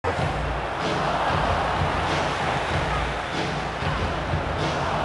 My Pro's - Torres-esk 30 Yard Curler!!!! Late Winner!!!!
My Pros 82nd Min Winner in a Online Clubs match (my pro looks like torres!)